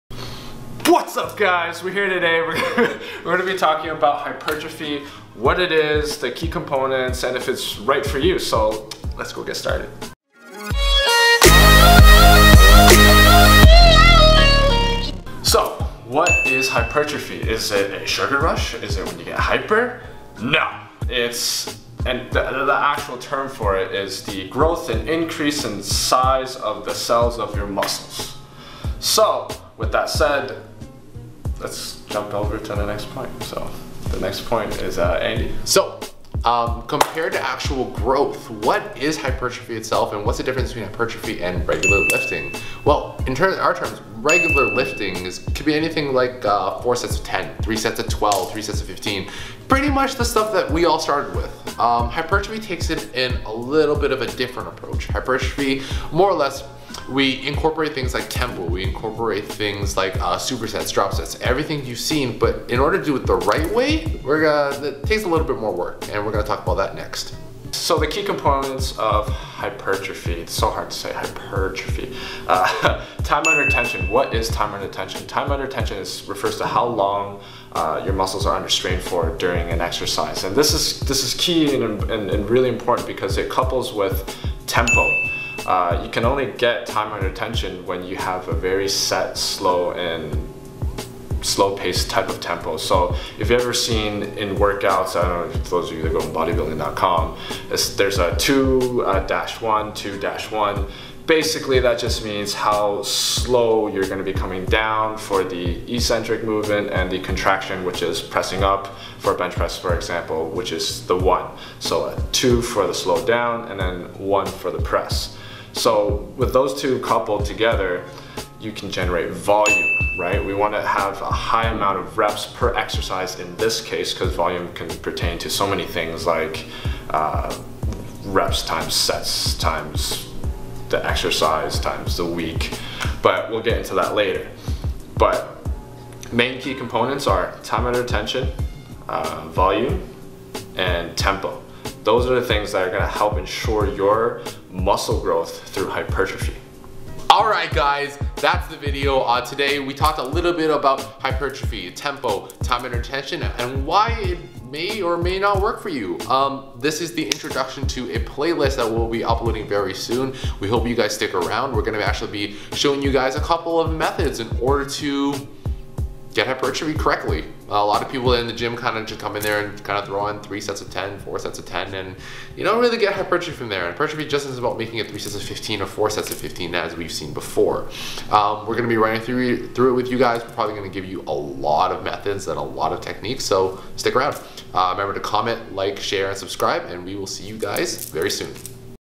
Intro to HYPERTROPHY
Hey guys!
Here’s an introduction to our new series about Hypertrophy and what it is.
In the upcoming weeks we’ll be talking about various hypertrophy methods and protocols which you guys can apply to your own workouts to achieve maximum results!

Let us know what you guys thought about this video; what you liked about it, what you didn’t like about it.

We’ll see you soon :)

Music we used